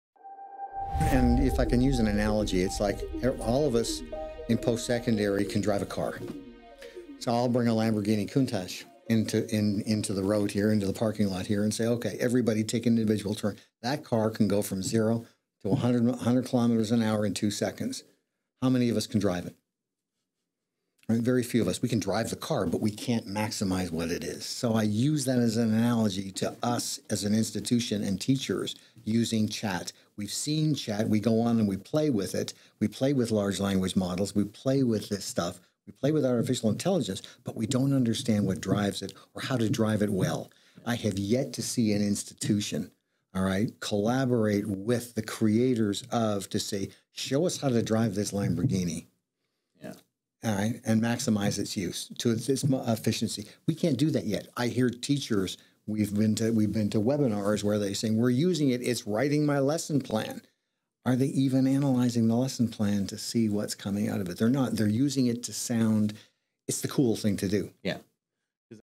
Maximizing the Potential of AI in Education | The New Intelligence Podcast
Educators and institutions have access to powerful tools like artificial intelligence and chatbots. However, the real question is, do we truly understand how to harness the full potential of these tools? 🤖📚

Join us in this thought-provoking discussion as we explore the analogy of driving a high-performance car to using AI and chatbots in education. Discover how collaboration with AI creators can help maximize efficiency and enhance the learning experience for students. 🧠✨